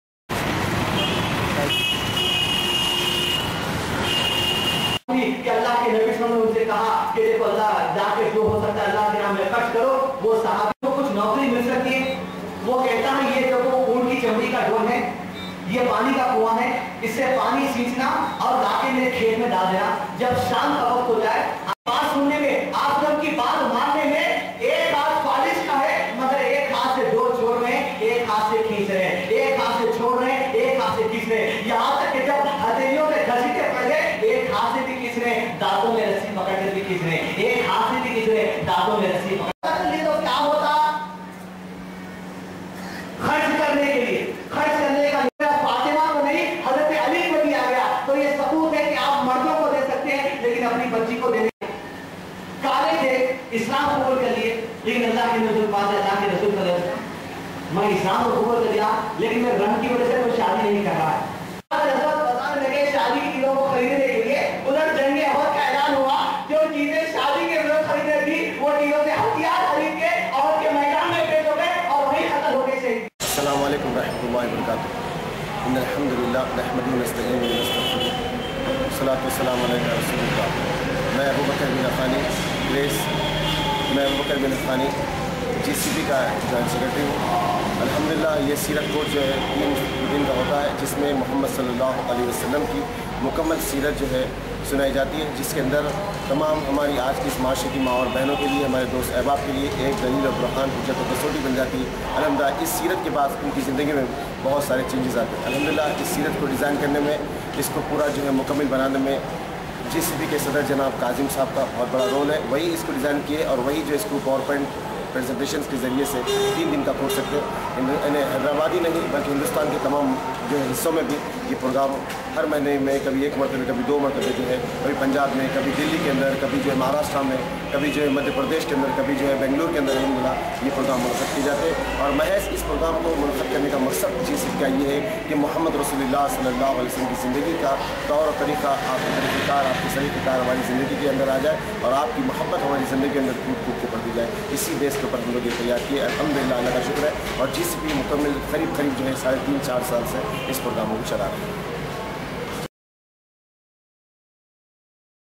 Newswala : 3 Day Seerat Course organized by GCP at Meridian Funcition Palace.
Newswala 3 Day Seerat Course organized by Guidence center for Peace (GCP) at Meridian Funcition Palace. Rayees Azam